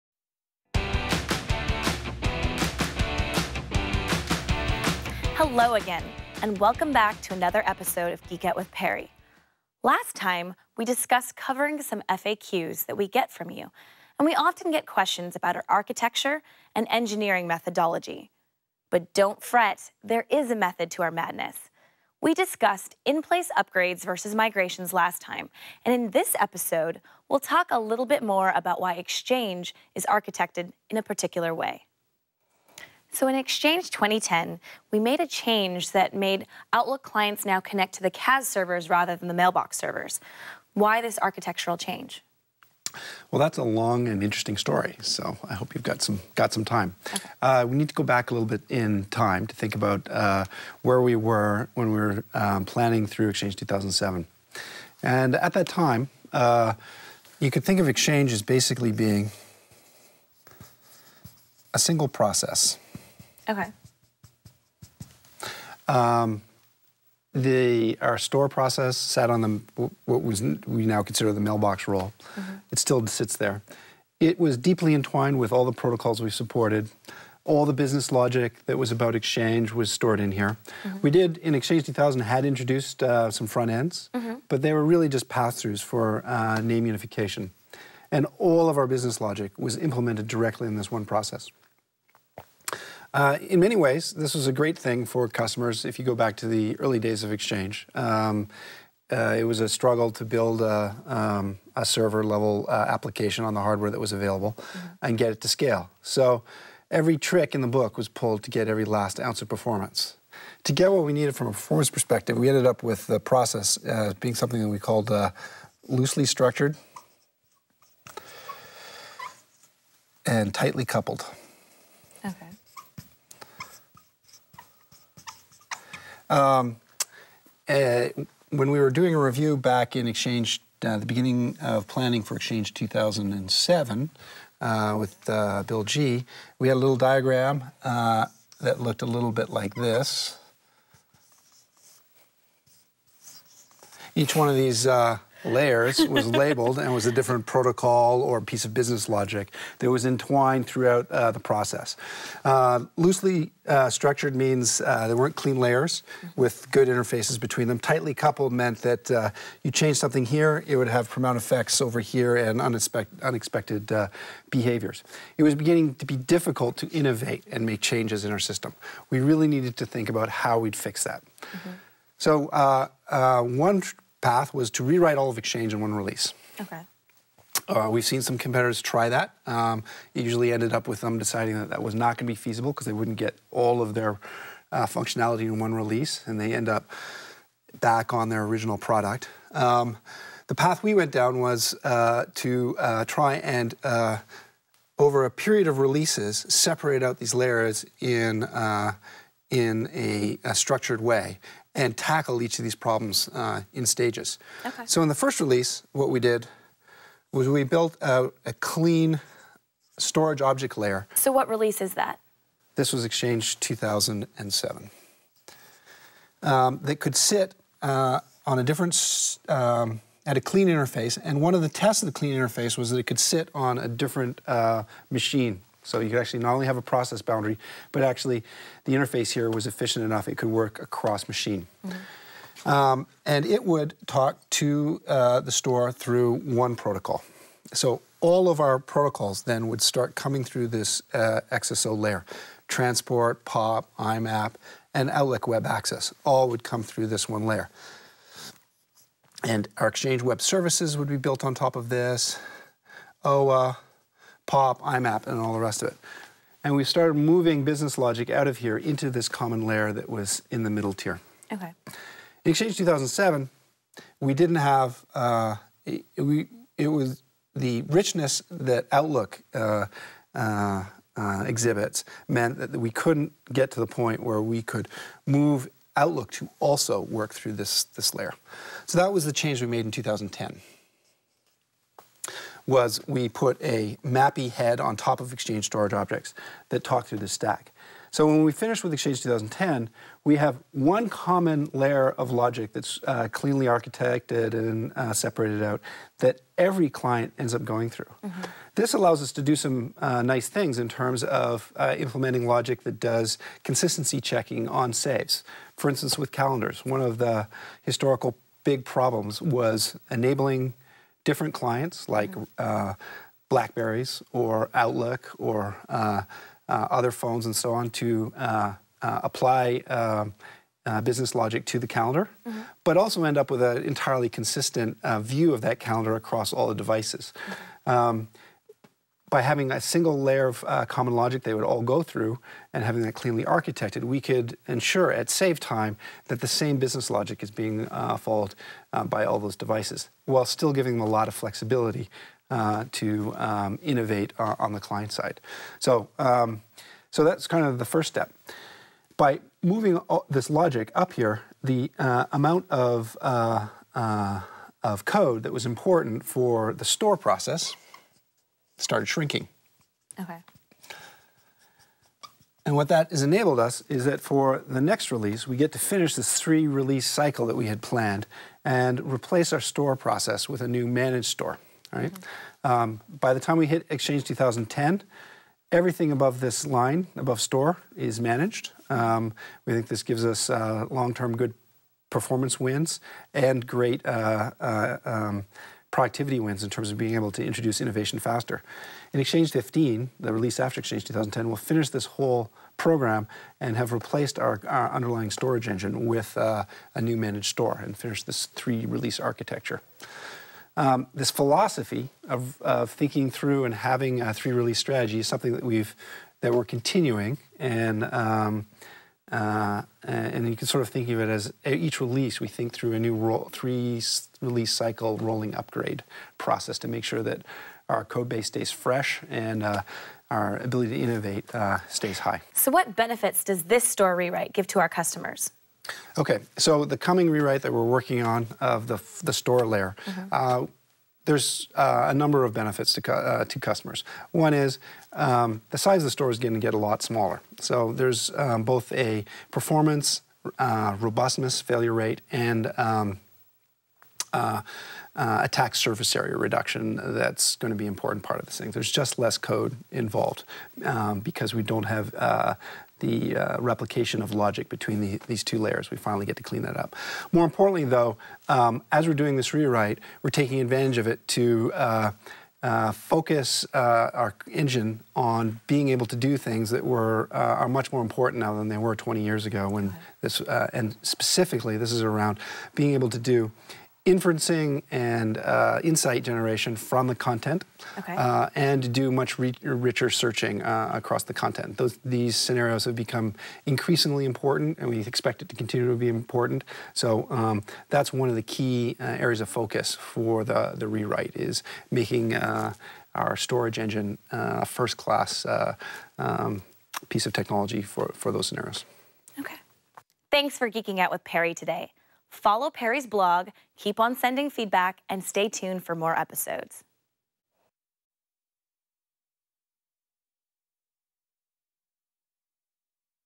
Geek Out with Perry on Exchange Architecture
Perry Clarke, Distinguished Engineer in Exchange discusses our architecture and engineering methodology and how Exchange has evolved over a few releases.